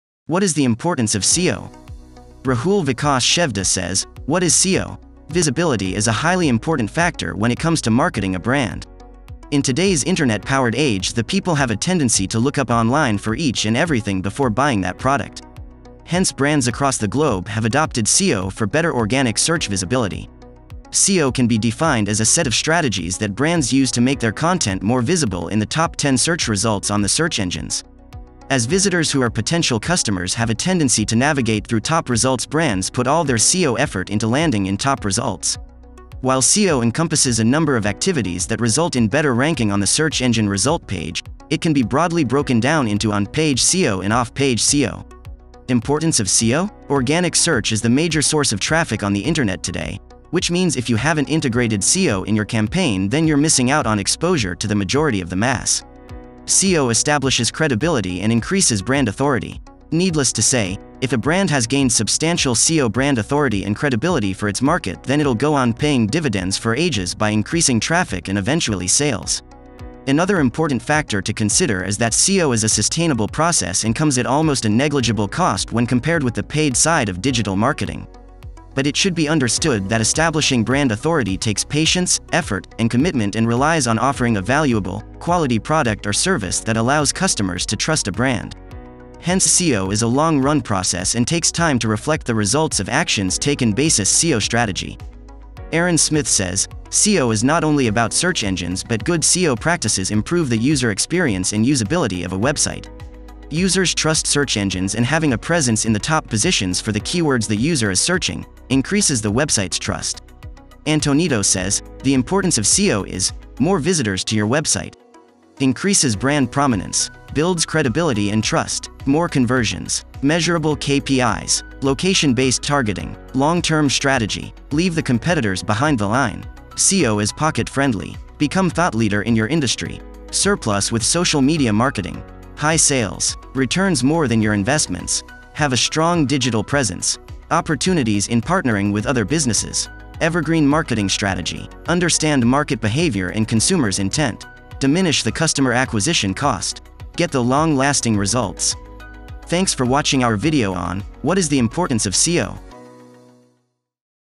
What is the importance of SEO?
Visibility is a highly important factor when it comes to marketing a brand. In today’s internet-powered age the people have a tendency to lookup online for each and everything before buying that product. Hence brands across the globe have adopted SEO for better organic search visibility.

SEO can be defined as a set of strategies that brands use to make their content more visible in the top 10 search results on the search engines. As visitors who are potential customers have a tendency to navigate through top results brands put all their SEO effort into landing in top results.

While SEO encompasses a number of activities that result in better ranking on the search engine result page, it can be broadly broken down into On-page SEO and Off-Page SEO.

Importance of SEO?

Organic Search is the major source of traffic on the Internet today, which means if you haven’t integrated SEO in your campaign then you’re missing out on exposure to the majority of the mass.

SEO establishes credibility and increases brand authority. Needless to say, if a brand has gained substantial SEO brand authority and credibility for its market then it’ll go on paying dividends for ages by increasing traffic and eventually sales! Another important factor to consider is that SEO is a sustainable process and comes at almost a negligible cost when compared with the paid side of digital marketing.

But it should be understood that Establishing brand authority takes patience, effort, and commitment and relies on offering a valuable, quality product or service that allows customers to trust a brand. Hence SEO is a long-run process and takes time to reflect the results of actions taken basis SEO strategy.

SEO is not only about search engines but good SEO practices improve the user experience and usability of a website. Users trust search engines and having a presence in the top positions for the keywords the user is searching, increases the website's trust.

The importance of SEO is:

More Visitors To Your Website.
Increases Brand Prominence.
Builds Credibility And Trust.
More Conversions.
Measurable KPIs.
Location-based Targeting.
Long Term Strategy.
Leave The Competitors Behind The Line.
SEO Is Pocket-Friendly.
Become Thought Leader In Your Industry.
Surplus With Social Media Marketing.
High Sales.
Returns More Than Your Investments.
Have A Strong Digital Presence.
Opportunities In Partnering With Other Businesses.
Evergreen Marketing Strategy.
Understand Market Behavior And Consumers’ Intent.
Diminish The Customer Acquisition Cost.
Get The Long Lasting Results.